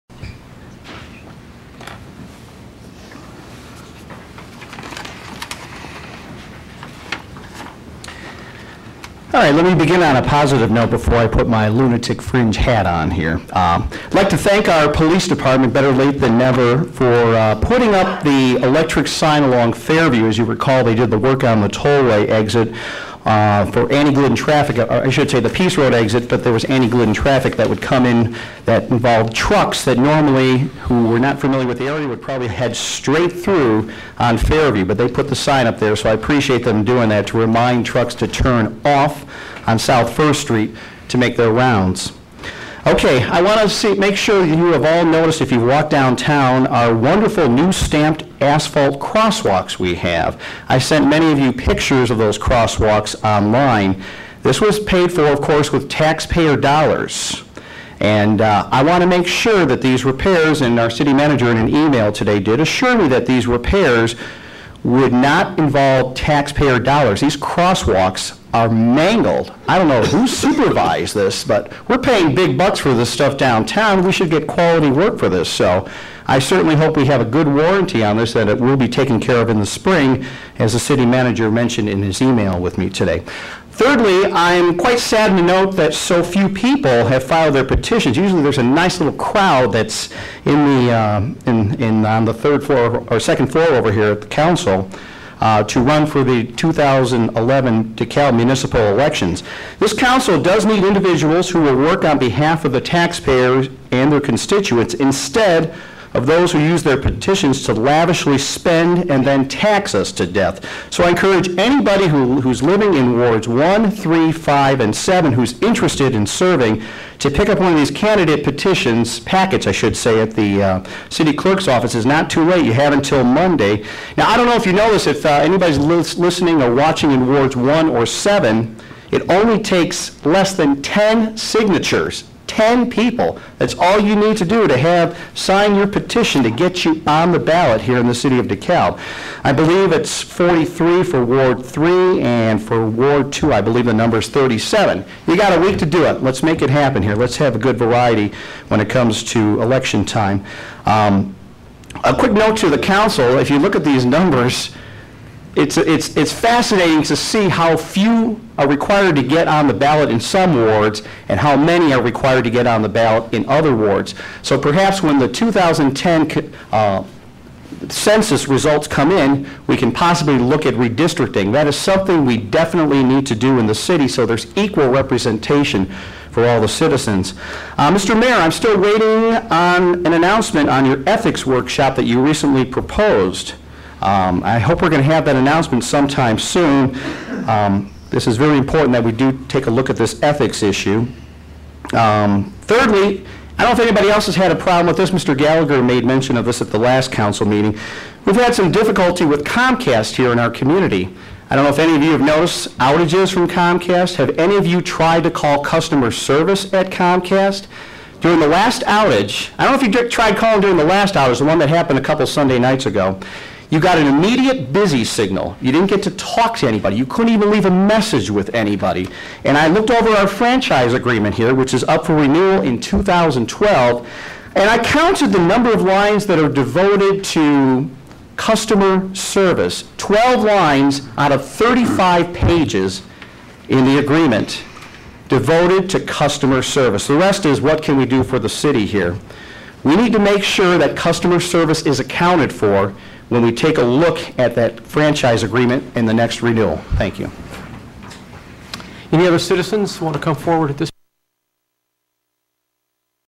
Dekalb IL Deteriorating Asphalt- NO Ethics Comcast Agreement
Citizen Comments 12-13-10: Deteriorating Stamped Asphalt, Mayor's Lack of his ethics commission,  Alderman Needed to Run for wards 1,3,5,7, Cable franchise  agreement needs to be reviewed...Very little mention of Customer service